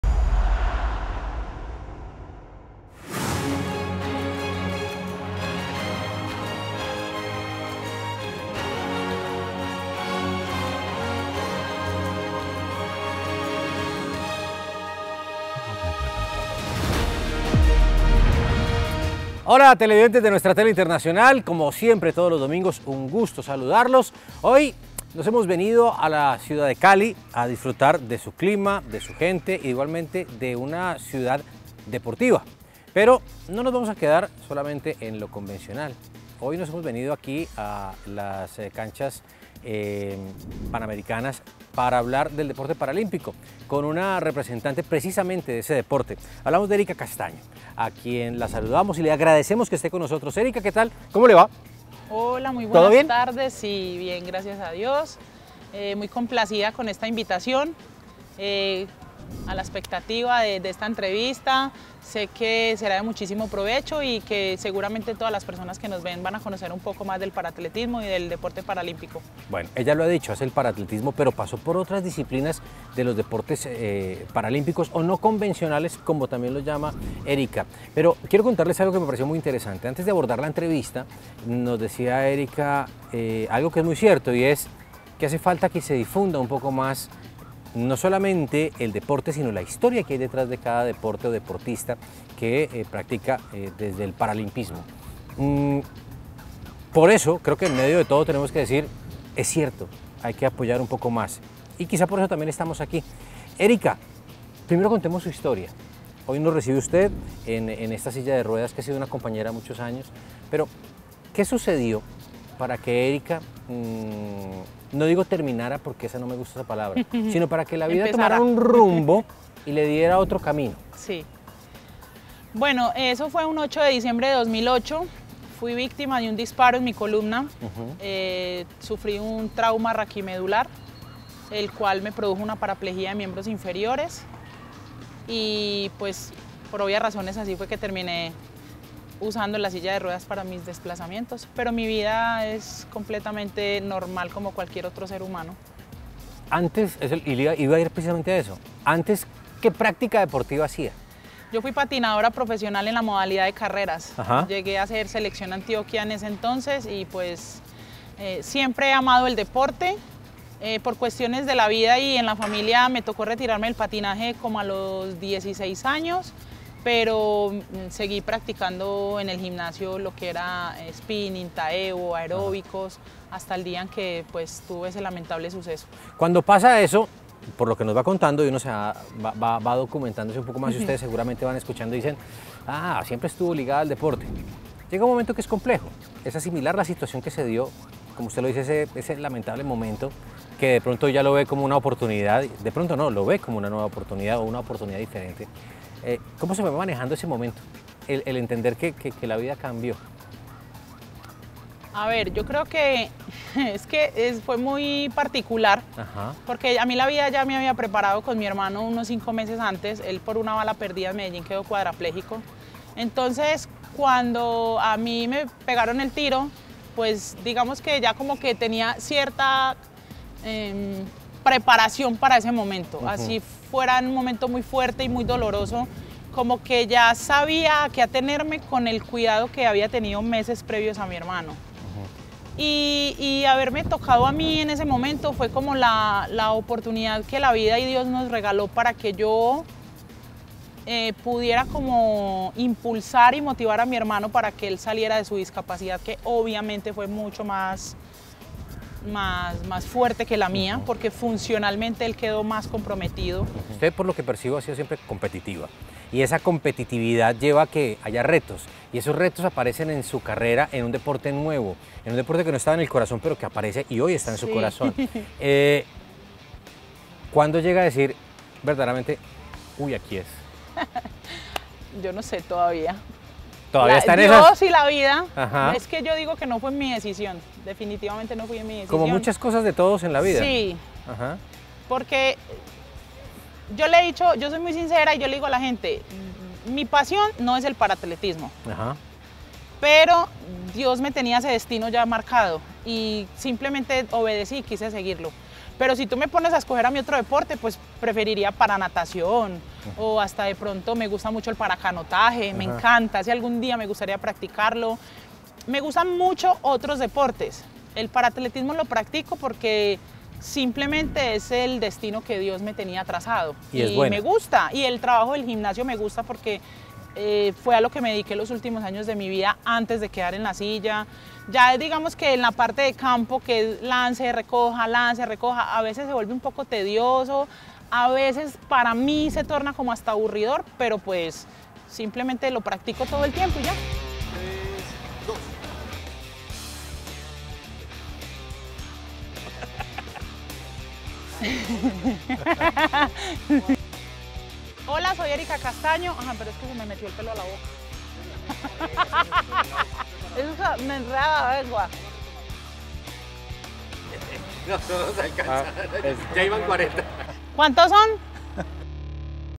Erica Castaño, la atleta paralímpica que conquistó el oro en París 2024
Erica Castaño es lanzadora de disco en la categoría F55 de paraatletismo colombiano con más de una década de trayectoria en la disciplina.

En 2008 pasó por uno de los momentos más complicados de su vida tras recibir un disparo que la dejó en silla de ruedas. Situación que le permitió descubrir nuevas habilidades y posicionarse como una fuerte representante.

Los Juegos Paralímpicos París 2024 fueron el escenario para conquistar el oro, un logro no solamente personal, sino también significativo para el país.

En una amena charla con José Fernando Neira, Erica nos contó sobre sus intereses deportivos, sus experiencias y retos. ¡Revive los mejores momentos de su entrevista a continuación!